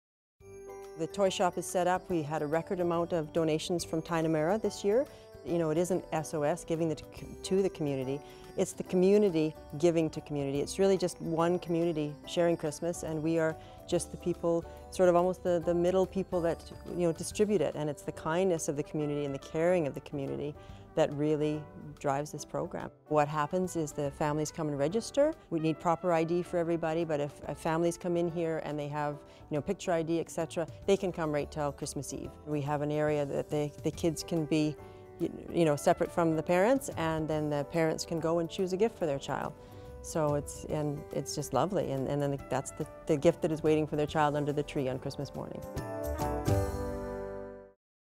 SOS Toy Shop Caring For Kids
The Society of Organized Services in Parksville allows families to shop for Christmas gifts for their kids free of charge.